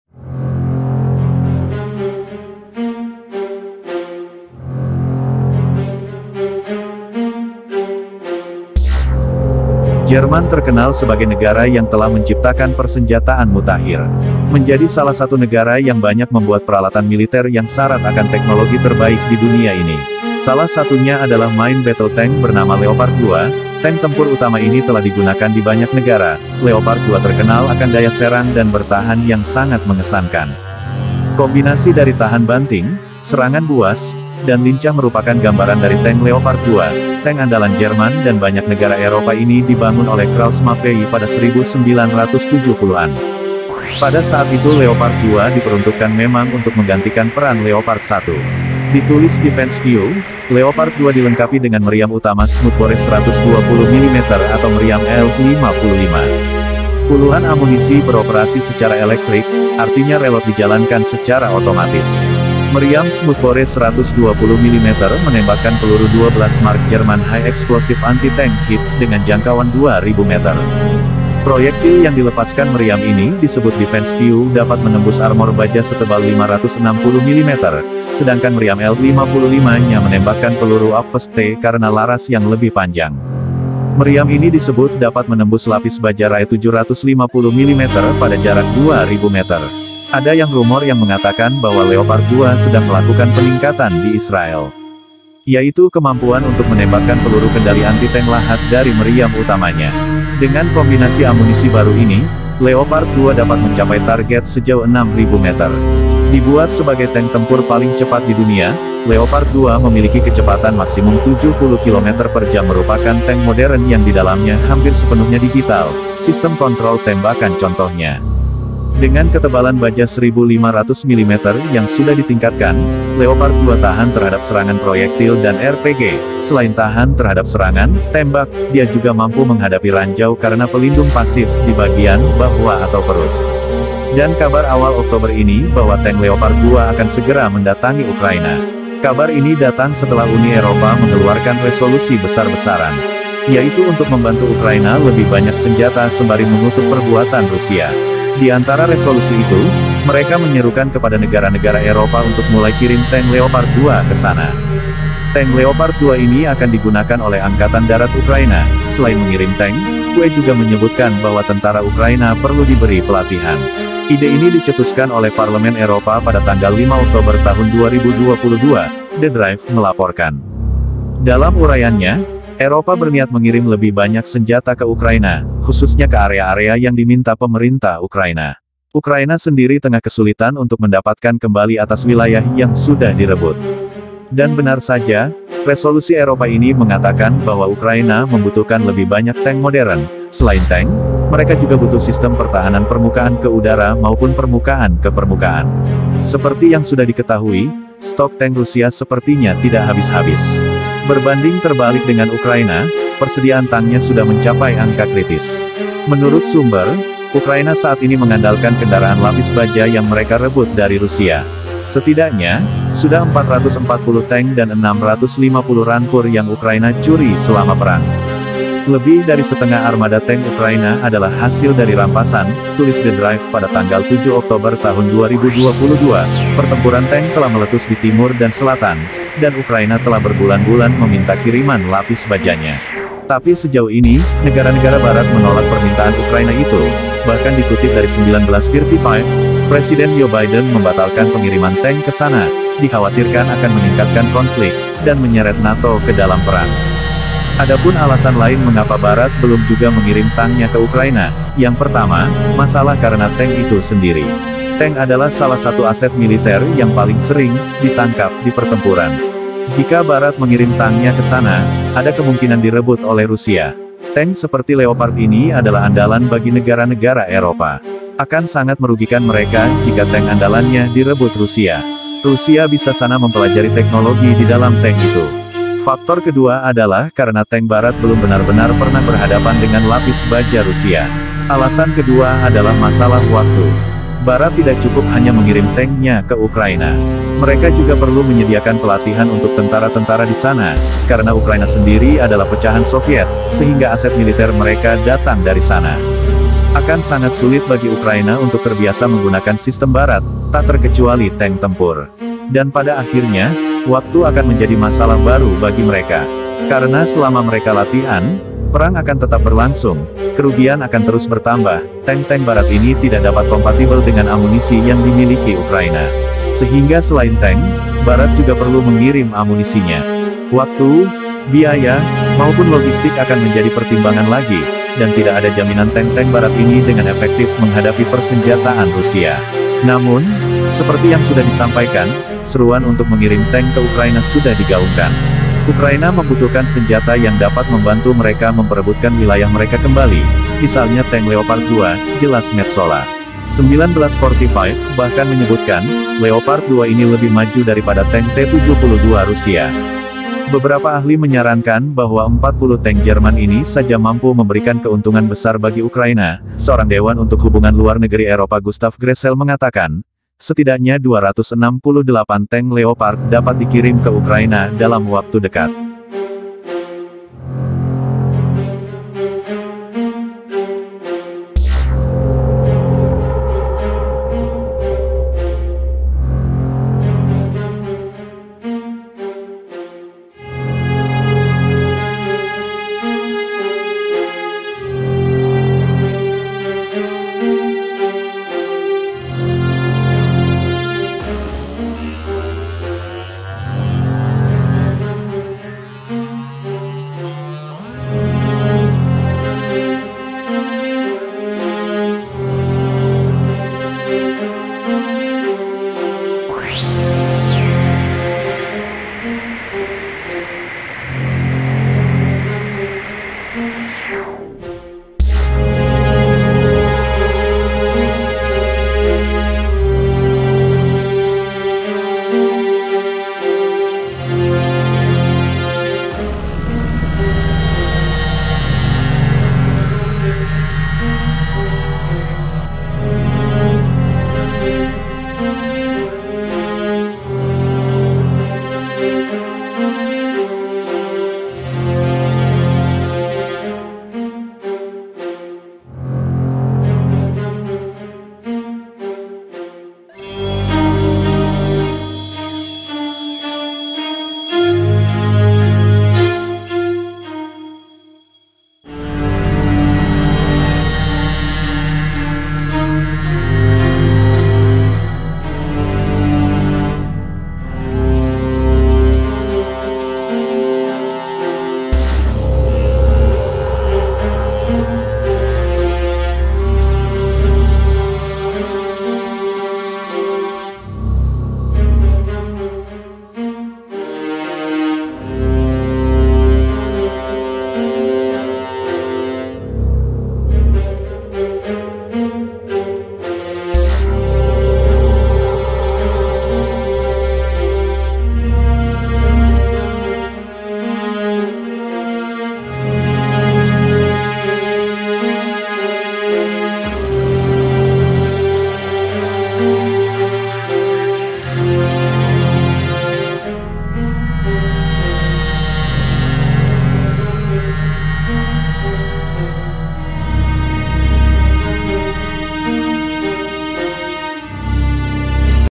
Digadang-gadang Sebagai Tank Terbaik Sepanjang Masa, | Leopard 2 Segera OTW Ke Ukraina
Jerman terkenal sebagai negara yang telah menciptakan persenjataan mutakhir.

Menjadi salah satu negara yang banyak membuat peralatan militer yang sarat akan teknologi terbaik di dunia ini.

Salah satunya adalah Main Battle Tank bernama Leopard 2.